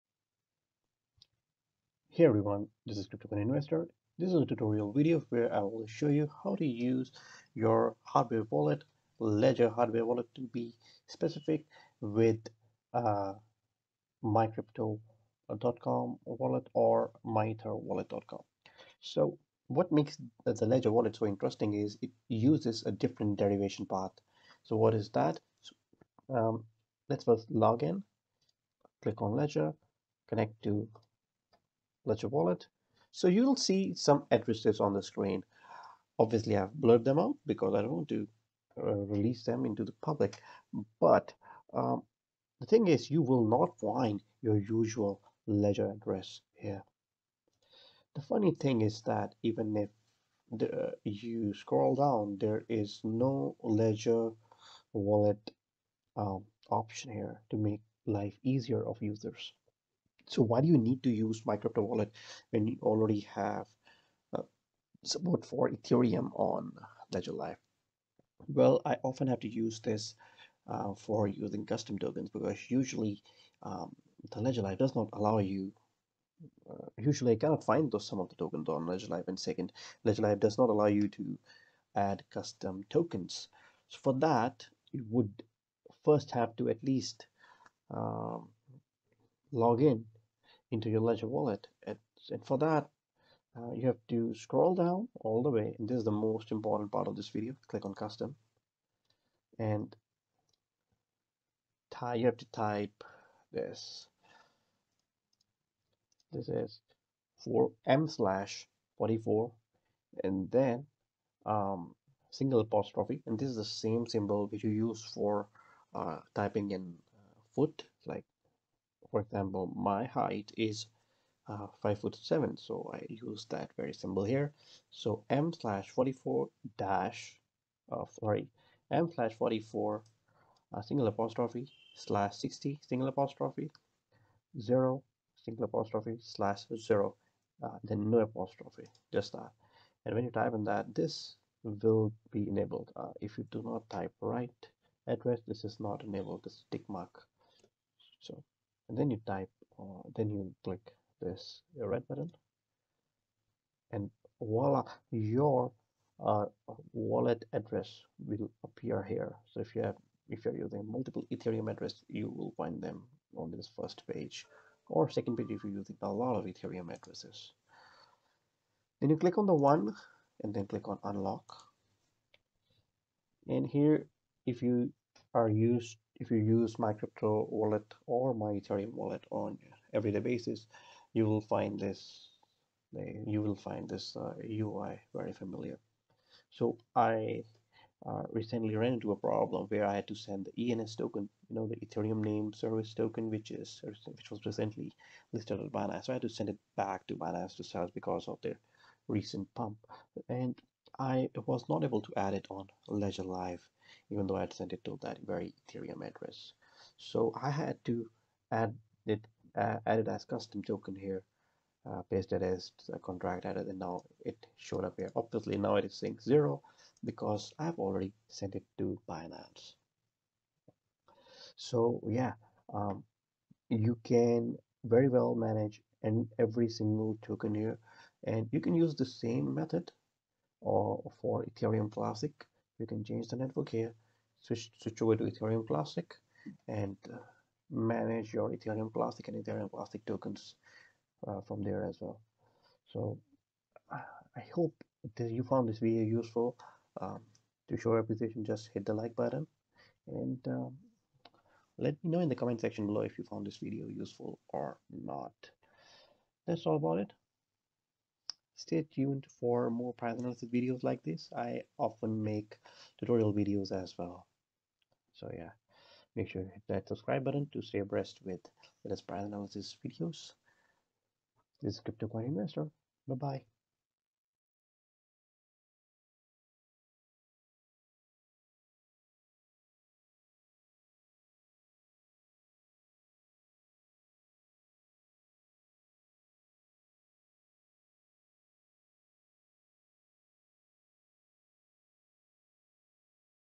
Using MyEtherWallet / MyCrypto with Ledger Nano S / X
How to use MyEtherWallet (or MyCrypto) wallet with Ledger Nano S or X.

1. Make sure you've already logged into Ethereum app on Ledger Nano S or X.
2. Enable "binding" in Ethereum app on Ledger Nano (find in settings).
3. Fire up MyCrypto or MyEtherWallet app and follow instructions given in video.

Derivation path m/44'/60'/0'/0

Everything said and shown in the video is my opinion. It is not official investment advice. Please do your own research before investing.